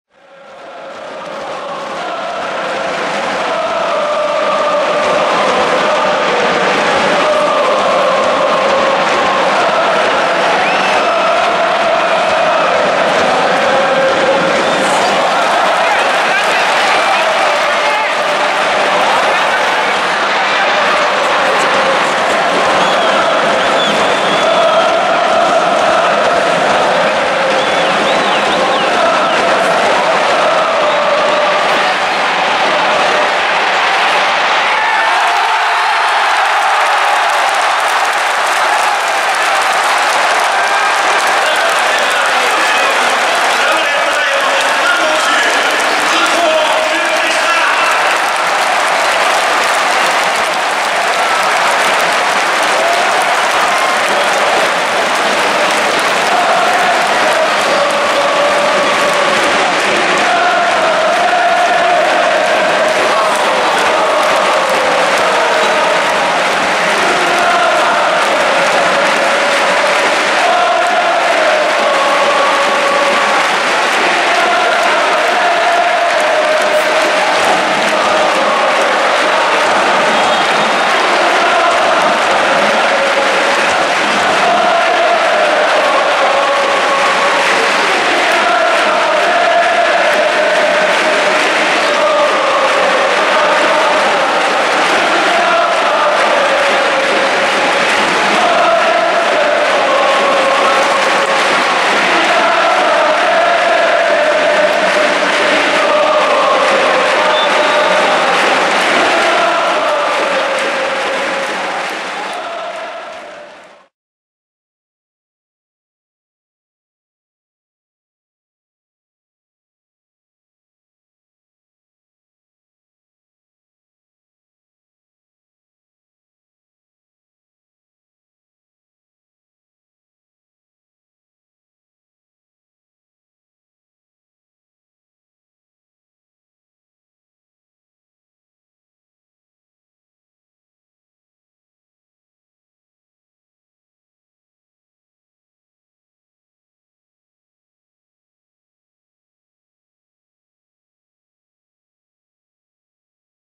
武藤雄樹ゴール 浦和レッズvsサンフレッチェ広島 2017第17節
45分　関根→柏木→
20170701　埼玉スタジアム